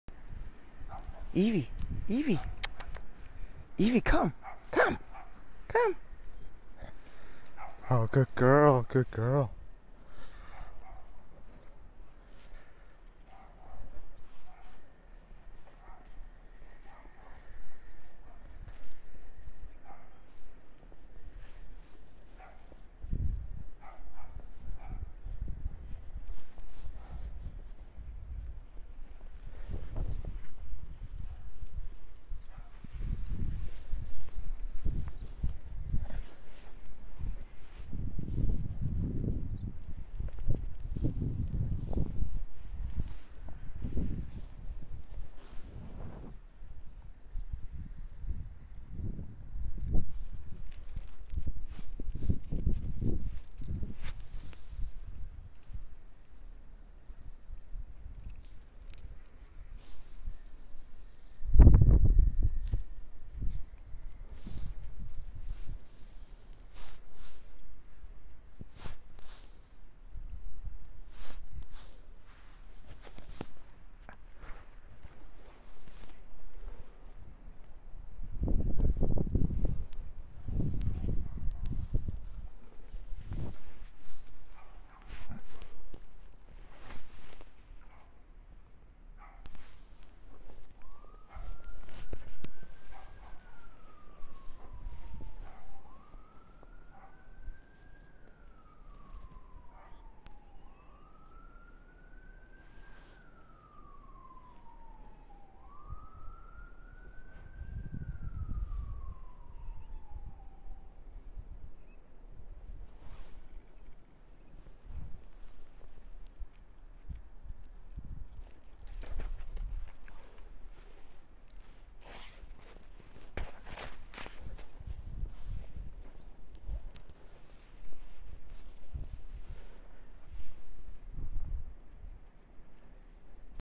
4 Month Old Puppy Playing in Snow After Blizzard of 2016 - Evie the Goldendoodle
Evie Doodle playing in the snow after the Blizzard of 2016.  If you love watching puppies running towards you, you'll want to start watching around the 1:55 mark.